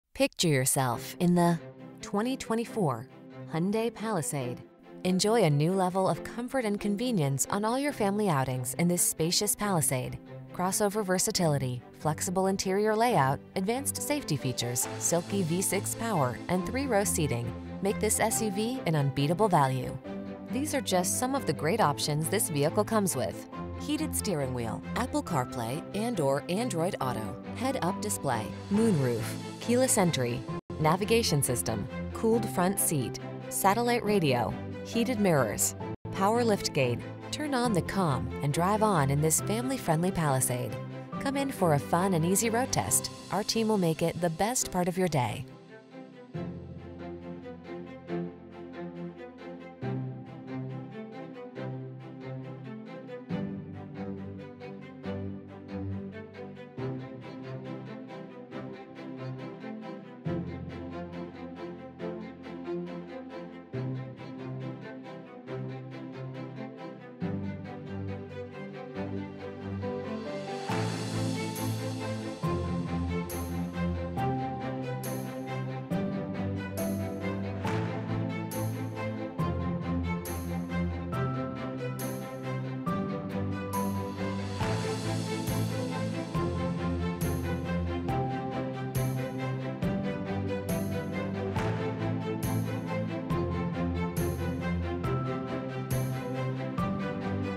2024 Hyundai Palisade Limited North Augusta, Aiken, Augusta, Grovetown, Evans SC
New 2024 Hyundai Palisade Limited available at Terry Lambert Hyundai located in 5585 Jefferson Davis Hwy, North Augusta, SC, 29841. Servicing the North Augusta, Aiken, Augusta, Grovetown, Evans area.

Picture yourself in the  2024  Hyundai Palisade. Enjoy a new level of comfort and convenience on all your family outings in this spacious Palisade. Crossover versatility, flexible interior layout, advanced safety features, silky V6 power, and 3-row seating make this SUV an unbeatable value. These are just some of the great options this vehicle comes with: Heated Steering Wheel
, Apple Carplay®/Android Auto®, Head-Up Display, Moonroof, Keyless Entry, Navigation System, Cooled Front Seat(s), Satellite Radio, Heated Mirrors, Power Liftgate. Turn on the calm and drive on in this family-friendly Palisade. Come in for a fun and easy road test. Our team will make it the best part of your day.